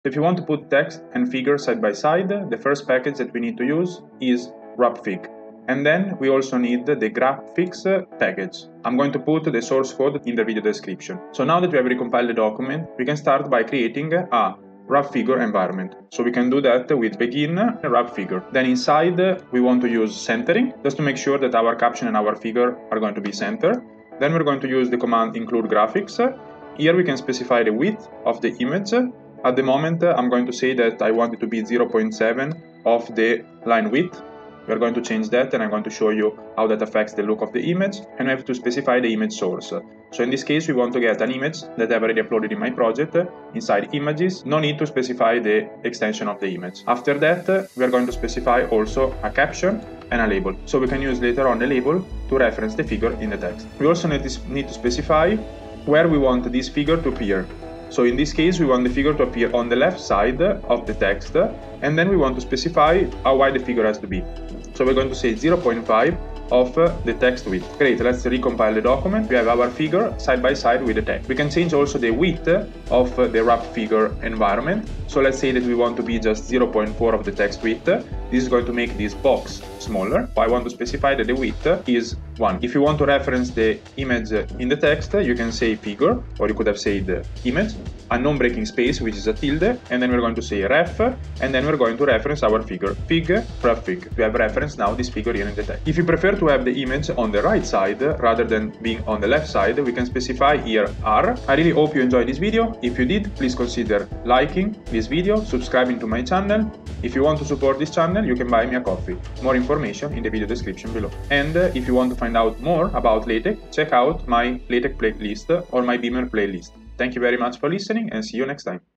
Wrapping text around a Figure - LaTeX in 100 seconds
Wrapping text around an image. Place text and figures side by side using the wrapfig package. Position an image on the left or right side of an image.

🔔 Click on the bell icon to be notified when I release a new video

📝 Useful notes:

👨‍💻 Source code:
\usepackage{wrapfig}
\usepackage{graphicx}

\begin{wrapfigure}{r}{0.4\textwidth}
\centering
\includegraphics[width=1\linewidth]{images/earth}
\caption{Caption}
\label{fig:wrapfig}
\end{wrapfigure}

🎥 Content of this video:
00:00 - What you will learn